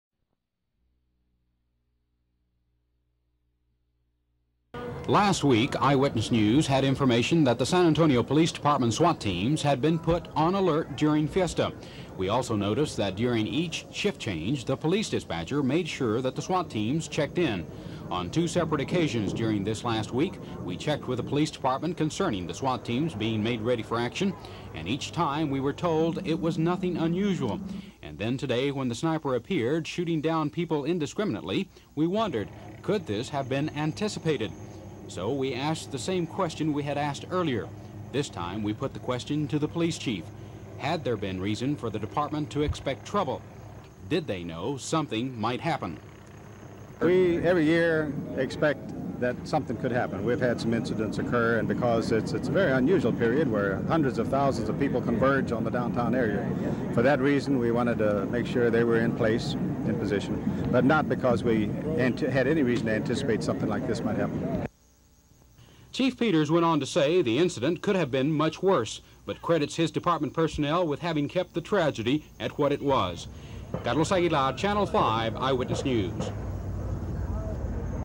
KENS 5 questions police chief on knowledge of sniper at Battle of Flowers Parade in 1979
KENS 5 receives news that the San Antonio SWAT things was put on alert for Fiesta. On two separate occasions, KENS 5 asked about the SWAT's presence and was told it was standard.